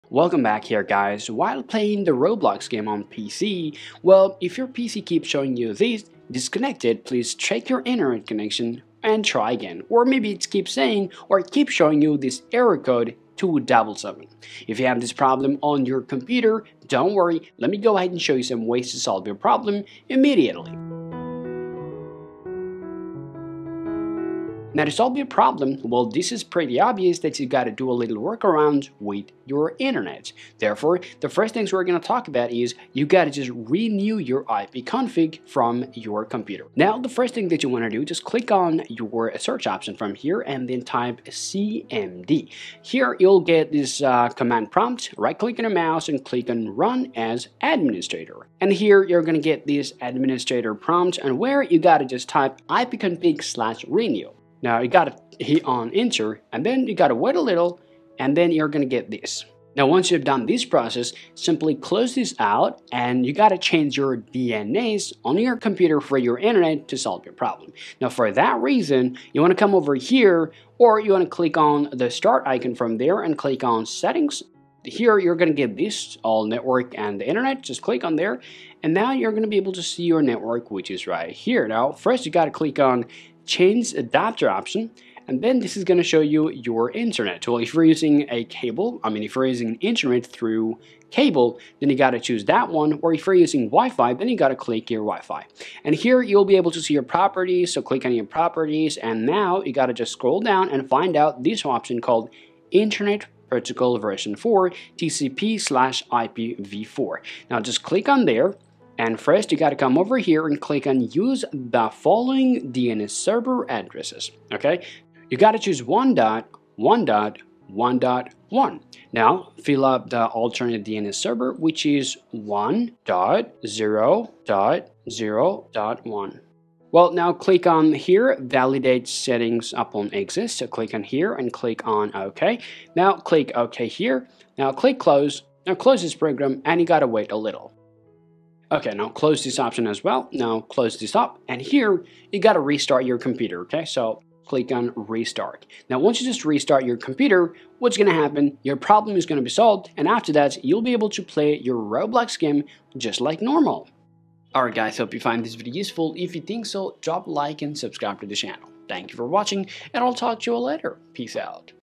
Error Code 277: Fix Roblox Disconnected Please Check Your Internet Connection And Try Again!
When you are playing Roblox Games on your Windows computer (Laptop/ Desktop) it is continuously showing you ‘Disconnect, Please Check Your Internet Connection and Try again, Error Code: 277'? This video tutorial will show you the 3 best Solutions to fix Roblox Error code 277 problem on Windows PC.

0:00 Opening
0:25 Solution 1: Renew iPconfig
1:02 Solution 2: Change DNS
2:21 Solution 3: Restart PC